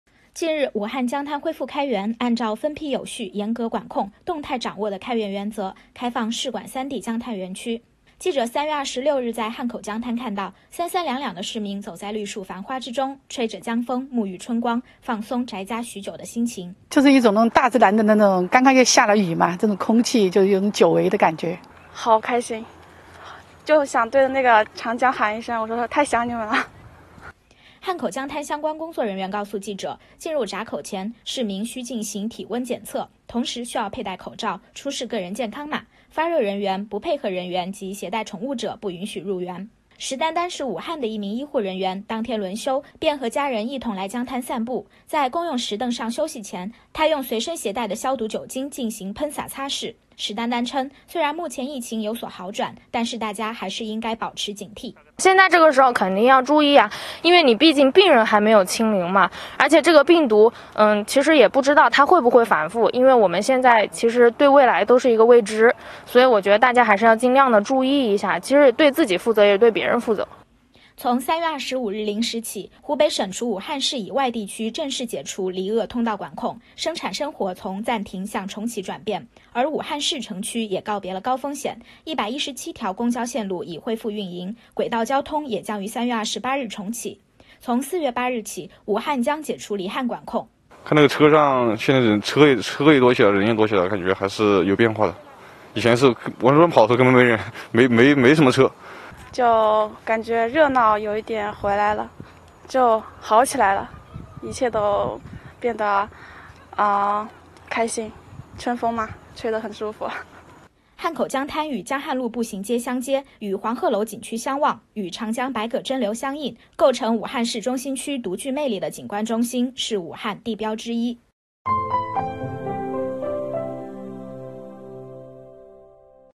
武汉江滩恢复开园 市民：久违了
近日，武汉江滩恢复开园，按照分批有序、严格管控、动态掌握的开园原则，开放市管三地江滩园区。